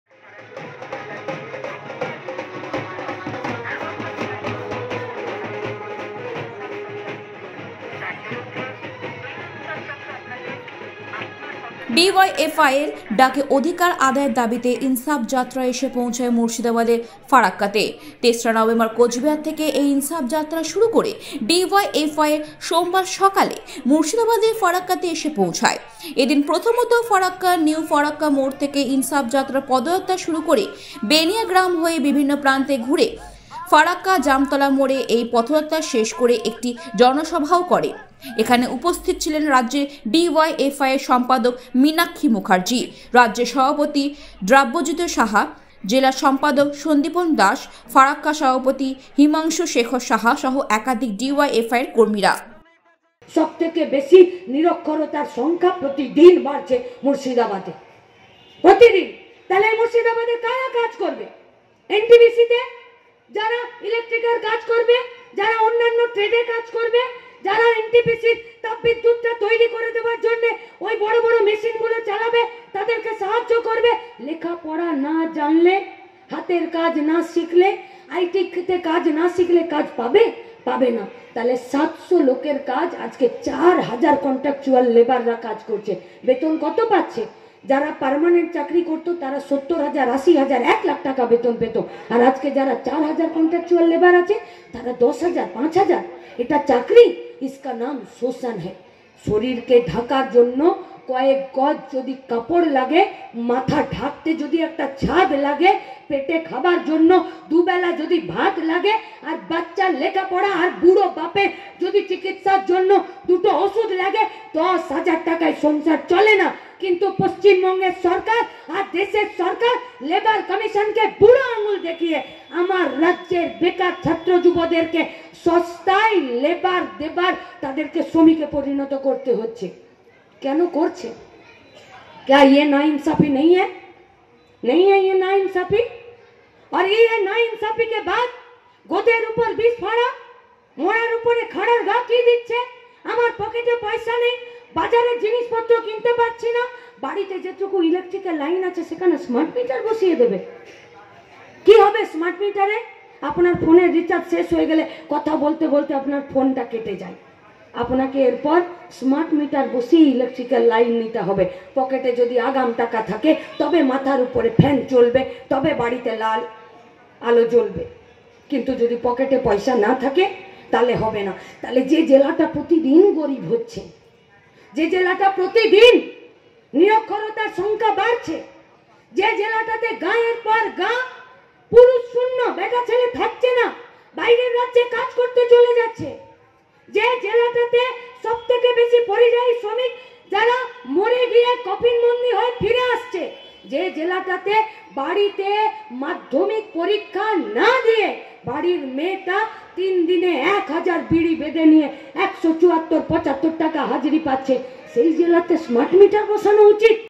DYFI Farakka: ডিওয়াইএফআই এর ডাকে অধিকার আদায়ের দাবিতে ইনসাফ যাত্রা ফরাক্কায়
এদিন প্রথমত ফরাক্কা নিউ ফরাক্কা মোর থেকে ইনসাফ যাত্রার পথযাত্রা শুরু করে বেনিয়াগ্রাম হয়ে বিভিন্ন প্রান্তে ঘুরে ফরাক্কা জামতলা মরে এই পথযাত্রা শেষ করে একটি জনসভাও করে।